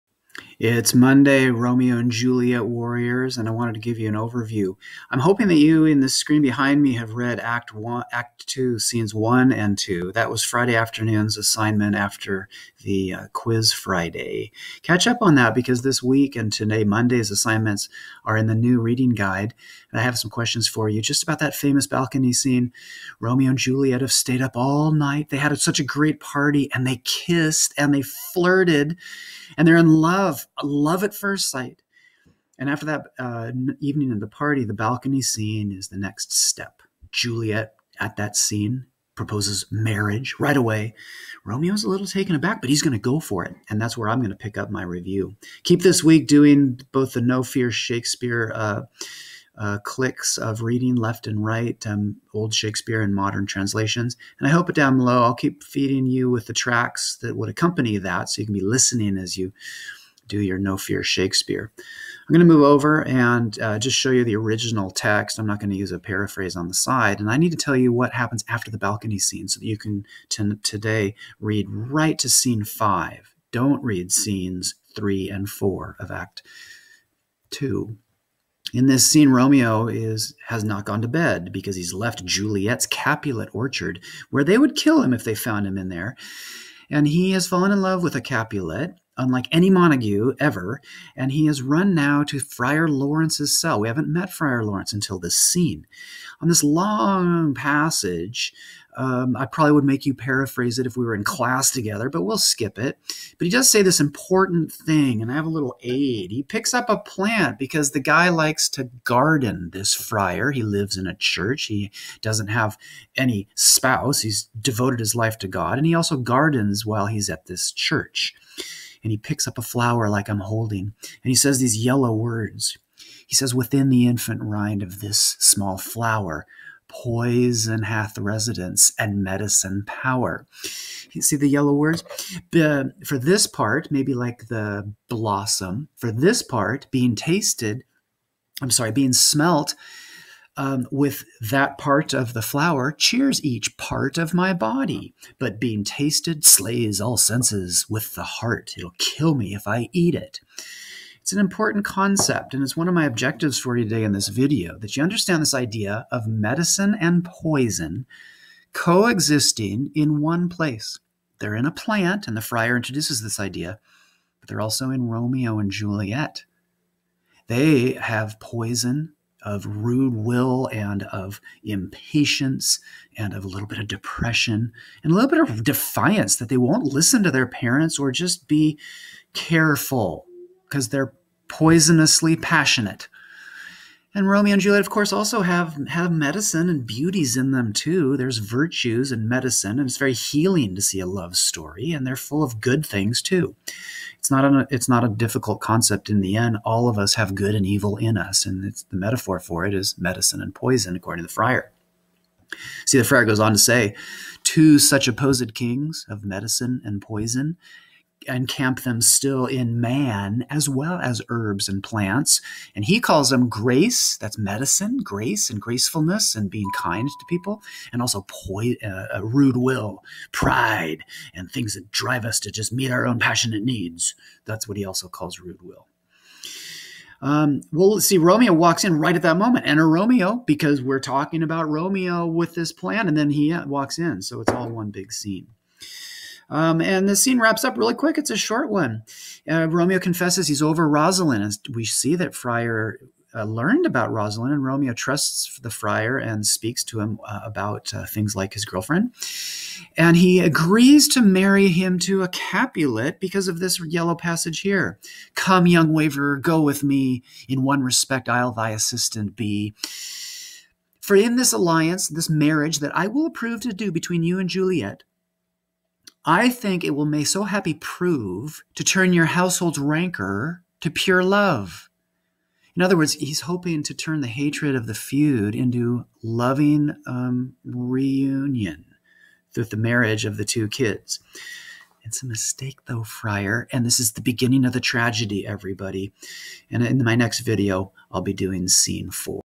R+J Summary Act 2 Scene 3
This 2.3 summary follows the famous balcony scene.